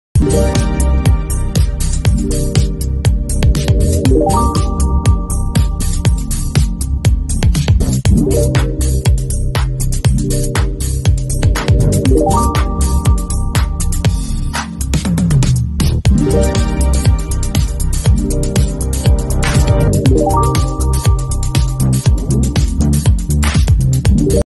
SNSC mini 300kg skid loader
Mini skid loader, 300kg load capacity, epa kubota engine, 0.2cbm bucket, more details please contact cecile freely.